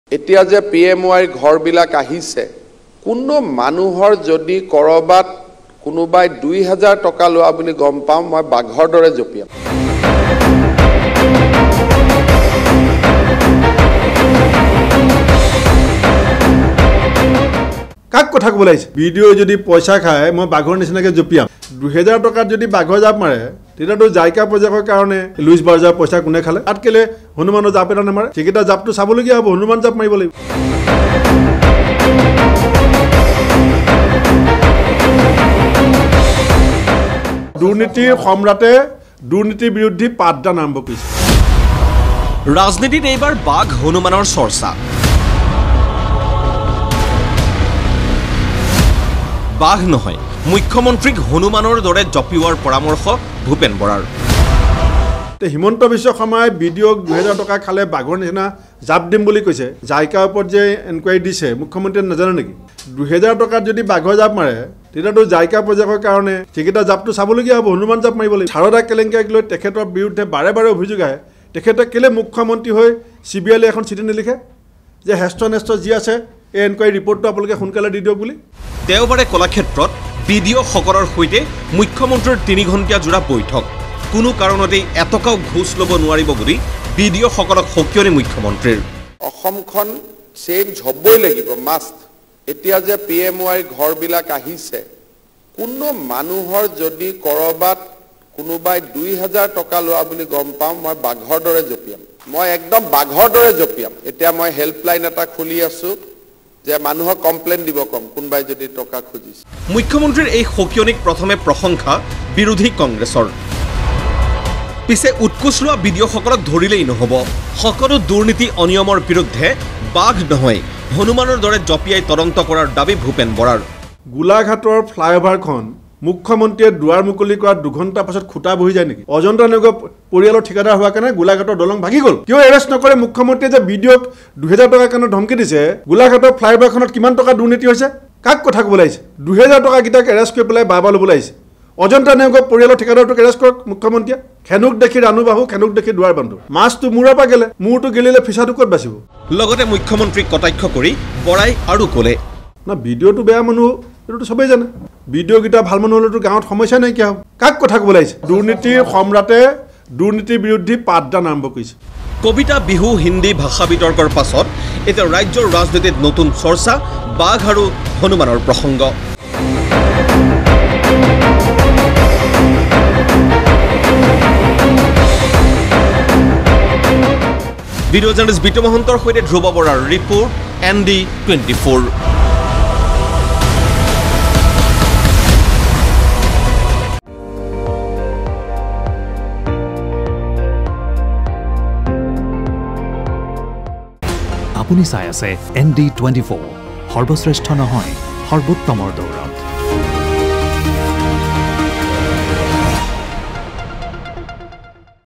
ৰাজনীতিত এইবাৰ বাঘ, হনুমানৰ চৰ্চা। বাঘ নহয়, মুখ্যমন্ত্ৰীক হনুমানৰ দৰে জঁপিওৱাৰ পৰামৰ্শ ভূপেন বৰাৰ
ঘোচ লব নোৱাৰিব। ঘোচ লোৱা কৰ্মচাৰীৰ ওপৰত বাঘৰ দৰে জঁপিয়াই পৰিব মুখ্যমন্ত্ৰী ড০ হিমন্ত বিশ্ব শৰ্মা। পিছে বাঘ নহয়, হনুমানৰ দৰে প্ৰতিটো দুৰ্নীতি অনিময়ৰ ক্ষেত্ৰত জঁপিয়াই পৰিবলৈ মুখ্যমন্ত্ৰীক আহ্বান ভূপেন বৰাৰ। পিছে কিয় এই পৰামৰ্শ মুখ্যমন্ত্ৰীগৰাকীক ভূপেন বৰাৰ? চাও আহক।